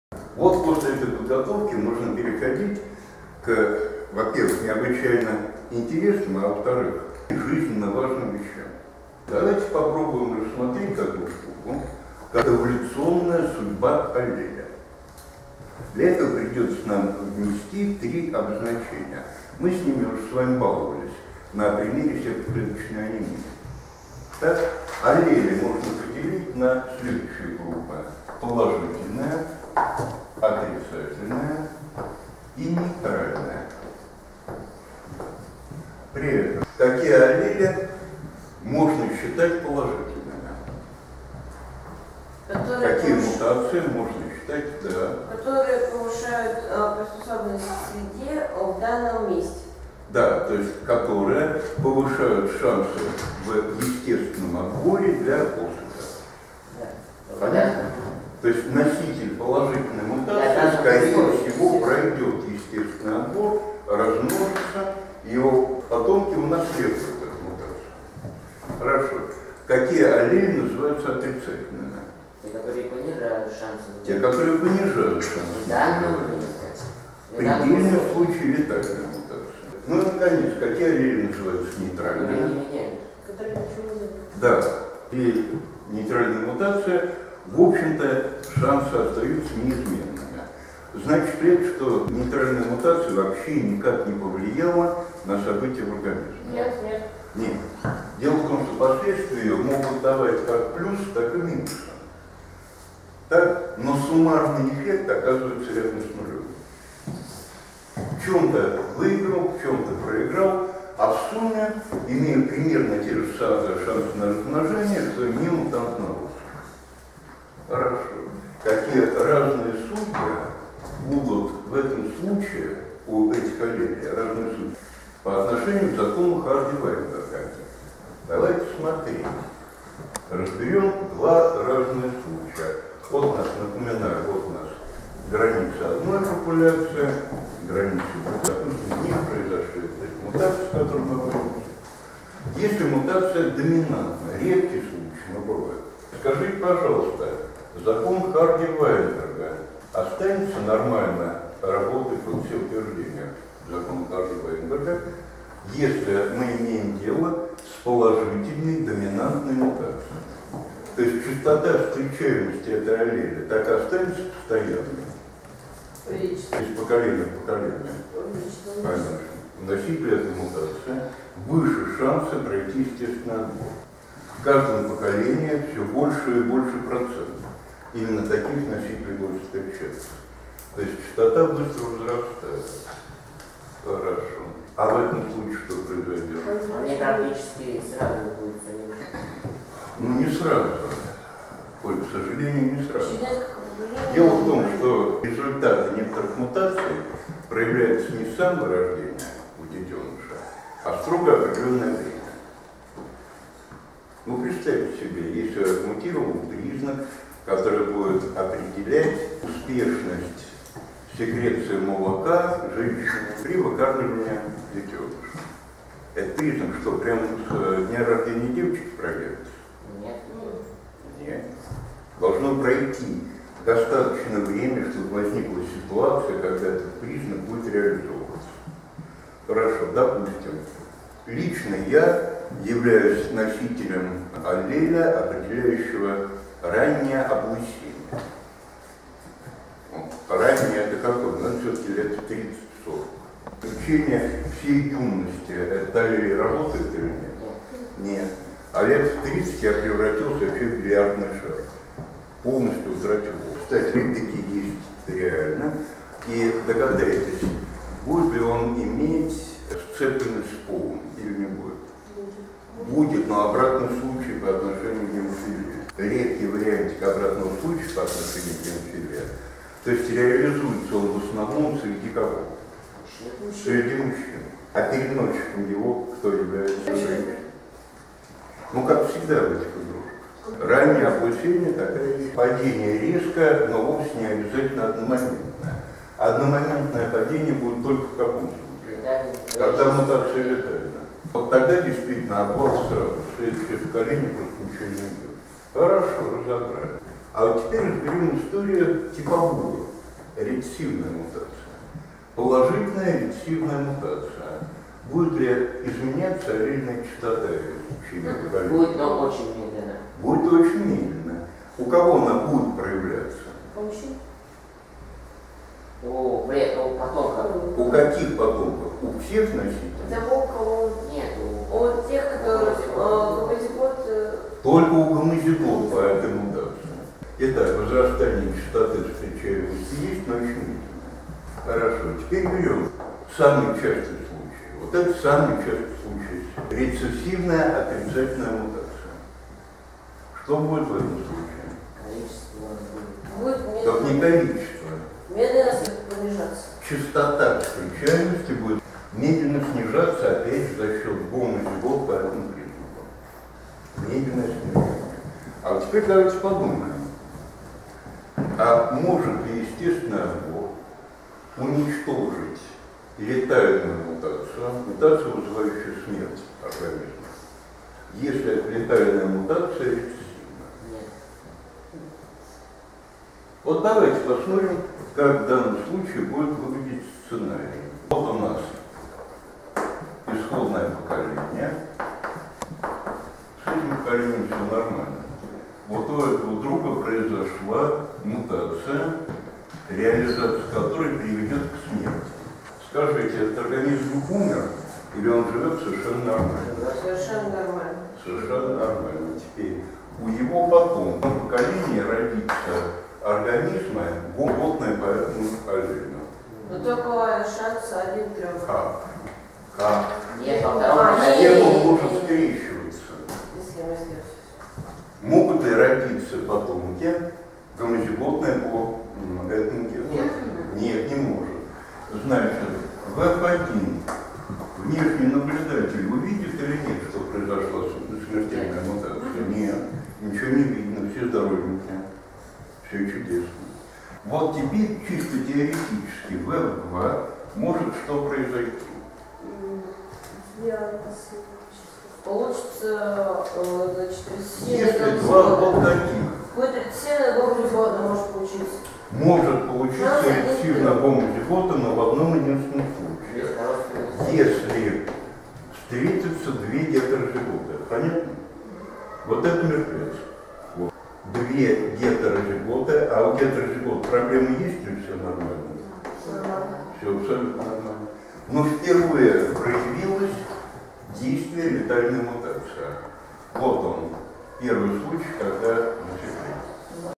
123 Эволюционная судьба аллеля
Положительные, отрицательные, нейтральные мутации. Летальная мутация. Судьба рецессивной отрицательной мутация.